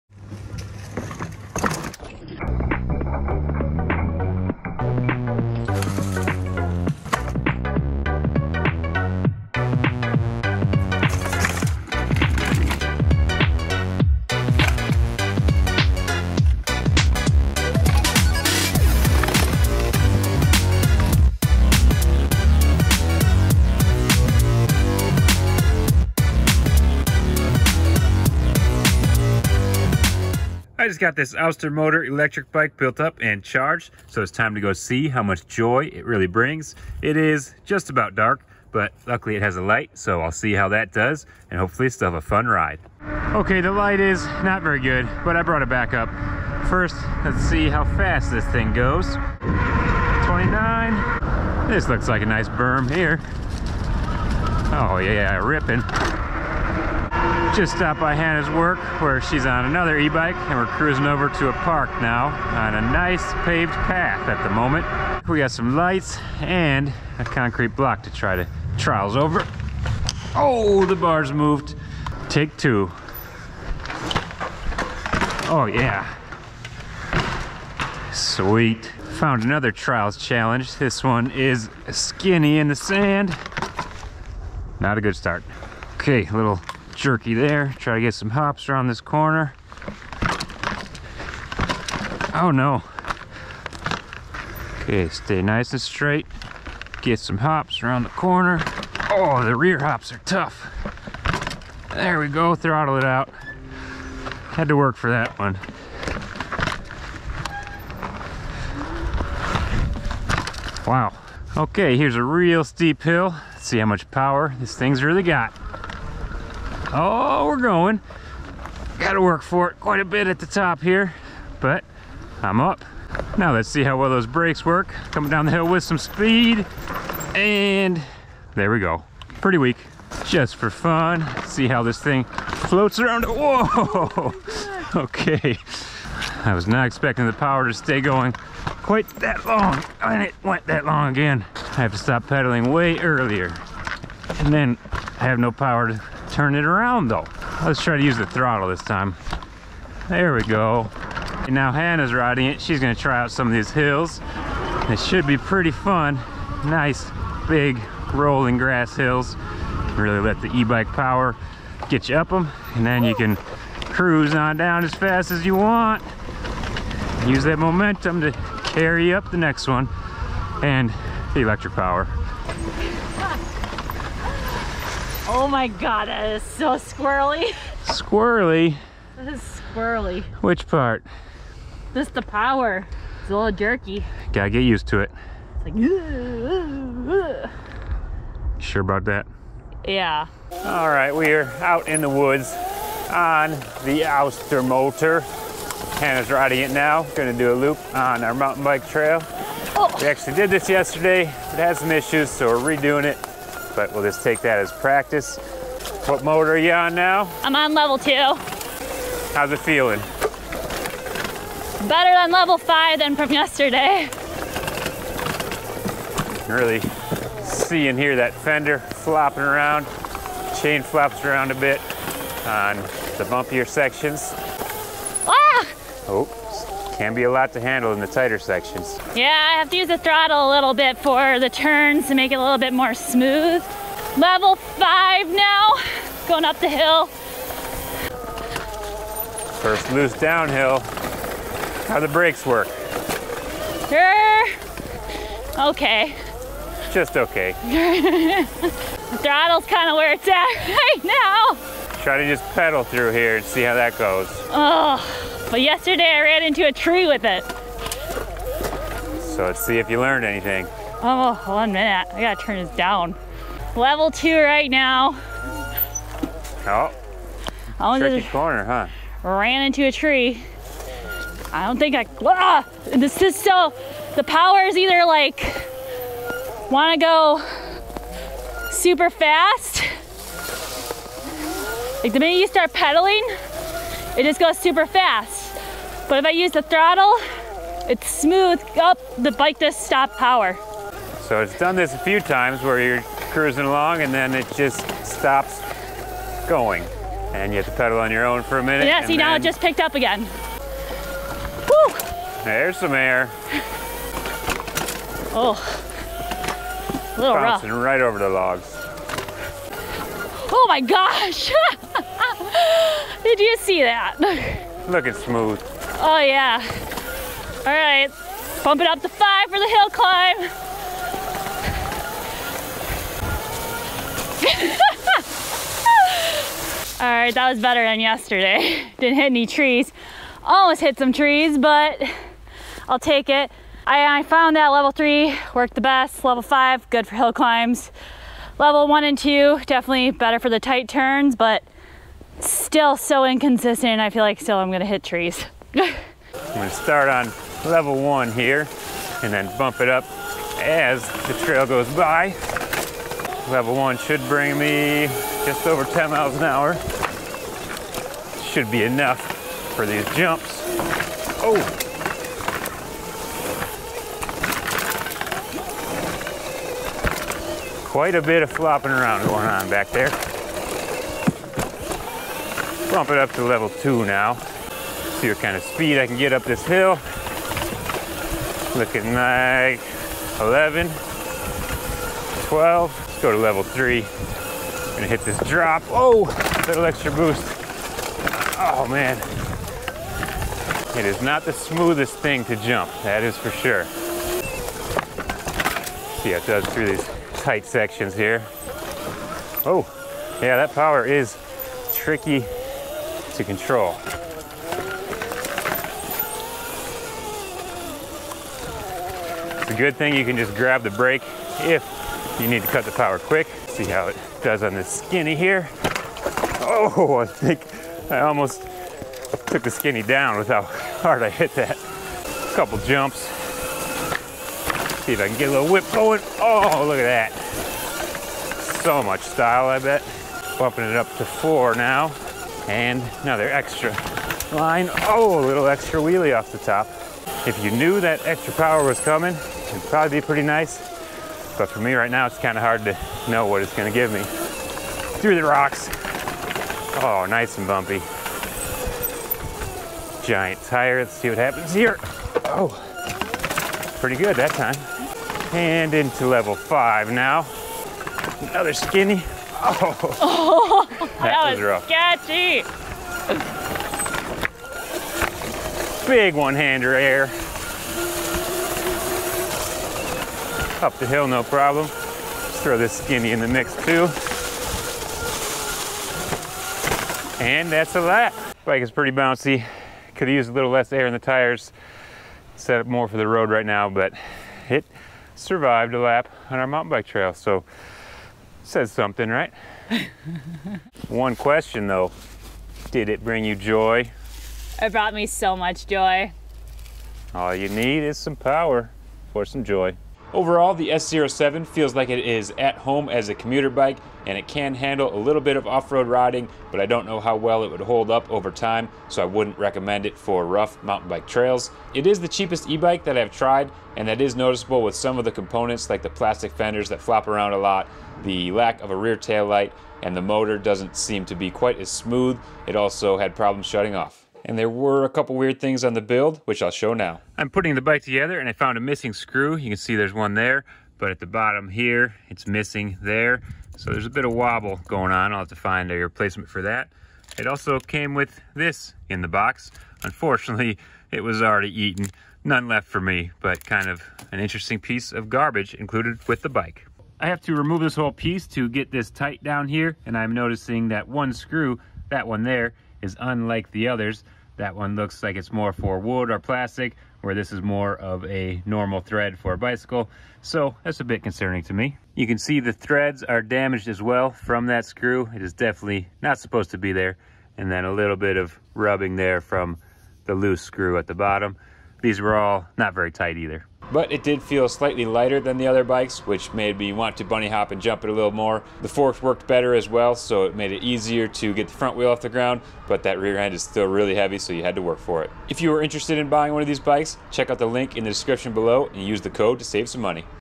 Road and Dirt Testing the Aostirmotor S07-B Electric Mountain Bike
Save $400 on this bike using code "AostirPat" until the end of December
The company has been contacted and will be working to make sure the screw and garbage problem will not happen again.